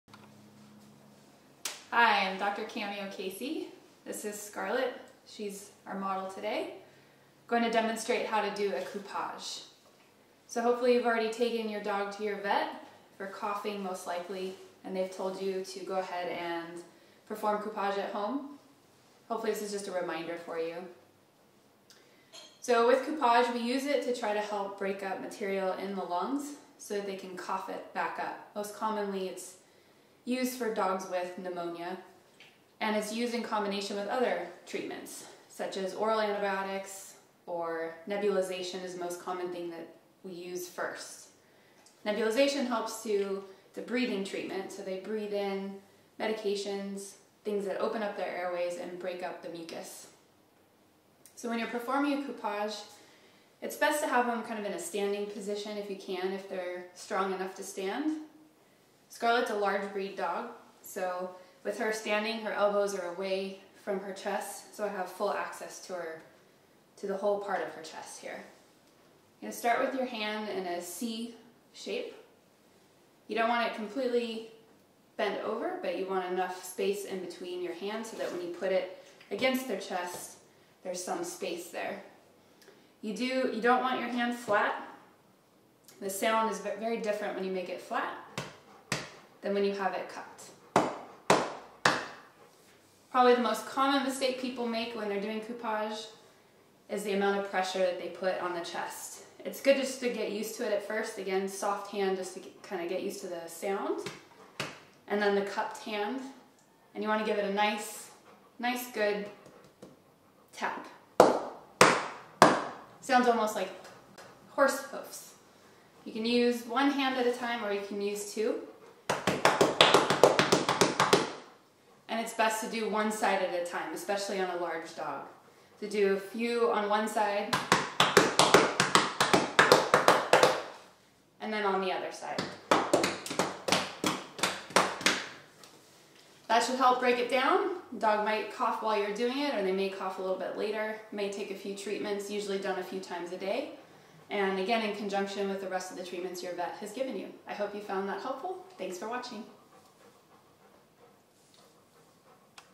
Canine coupage by Dr. Kameo
Dr. Kameo shows how to perform coupage on your dog at home. Please consult your veterinarian if your dog has breathing problems or is coughing.